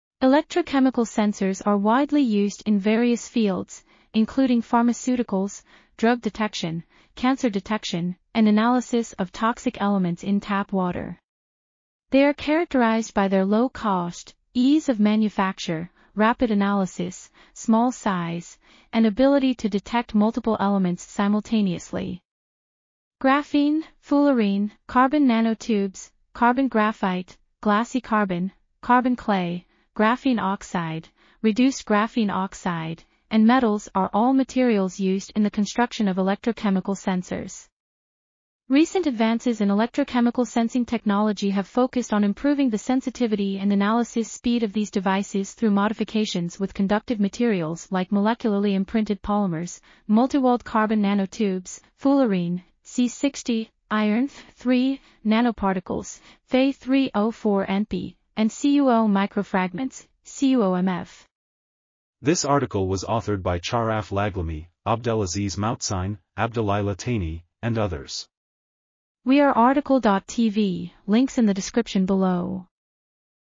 Recent advances in electrochemical sensors and biosensors for monitoring drugs and me... | RTCL.TV
### Keywords ###

### Article Attribution ###
Title: Recent advances in electrochemical sensors and biosensors for monitoring drugs and metabolites in pharmaceutical and biological samples
Authors: Charaf Laghlimi, Abdelaziz Moutcine, Abdelilah Chtaini, Jalal Isaad, Adil Soufi, Younes Ziat, Hassan Amhamdi ,and Hamza Belkhanchi
Publisher: International Association of Physical Chemists (IAPC)
DOI: 10.5599/admet.1709

### Video Timestamps ###
0:00:00 - Summary
0:01:02 - Title
0:01:09 - Outro
0:01:13 - End